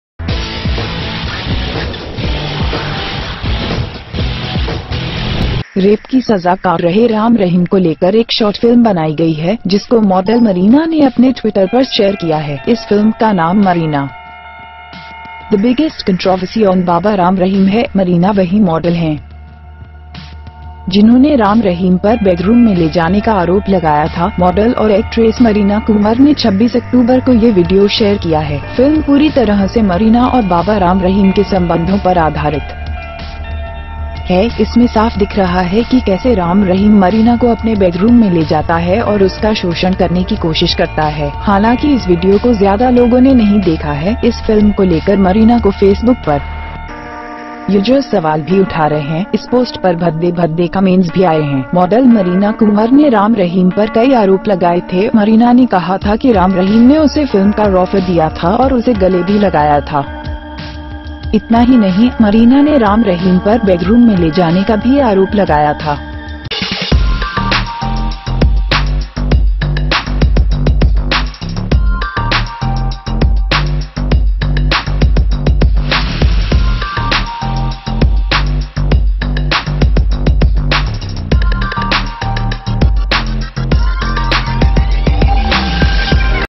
जिस मॉडल को राम रहीम ने बेडरूम में बुलाया था, उसने बलात्कारी बाबा पर बना दी है फिल्म
A short film has been made about Ram Rahim, who is serving a rape sentence. Whom the model Marina shared on Twitter. The name of this movie is Marina - The biggest Controversy on Baba Ram Rahim. Marina is the same model, who accused Ram Rahim of being in the bedroom.
The model and actress Marina Kunwar shared this video on October 26. The film is entirely based on the relationship between Marina and Baba Ram Rahim.
It is clearly visible in how Ram Rahim takes the marina to her bedroom, and tries to exploit it.
However this video has not been seen by more people. Marina is also raising user questions on Facebook about this movie. There are also bad-cut comments on this post.
A user has taken the model Marina to the target. He has written that there is still some shame, then take back your statement. Otherwise there will be no room for concealment.
One has written that O Shrishakari is flowing, your clothes are flowing through the dirty stream of your sacrament. Old age, marina aunt yours
Model Marina Kunwar had made several allegations against Ram Rahim. Marina had said that Ram Rahim offered him the film. And he had embraced it too. Not only this, Marina also accused Ram Rahim of being in the bedroom.